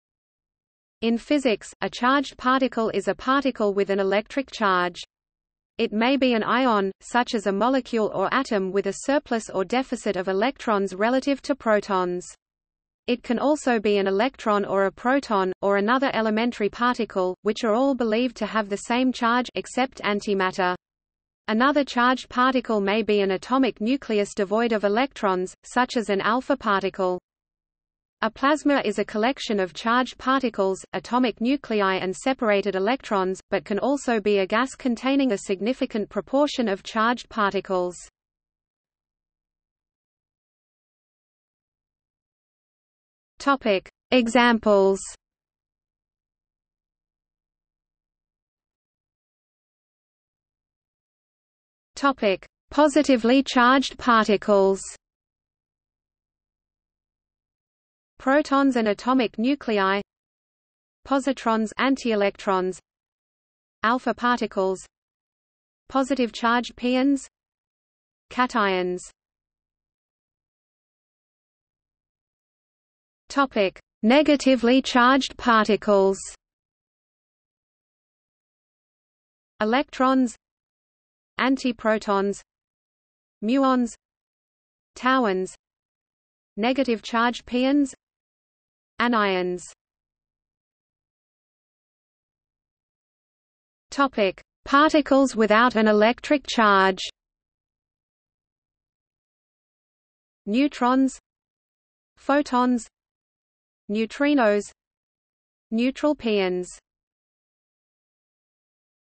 Charged particle | Wikipedia audio article
00:00:39 1 Examples
00:00:48 1.1 Positively charged particles
00:01:10 1.2 Negatively charged particles
00:01:30 1.3 Particles without an electric charge

- Socrates

SUMMARY
In physics, a charged particle is a particle with an electric charge. It may be an ion, such as a molecule or atom with a surplus or deficit of electrons relative to protons. It can also be an electron or a proton, or another elementary particle, which are all believed to have the same charge (except antimatter). Another charged particle may be an atomic nucleus devoid of electrons, such as an alpha particle.
A plasma is a collection of charged particles, atomic nuclei and separated electrons, but can also be a gas containing a significant proportion of charged particles.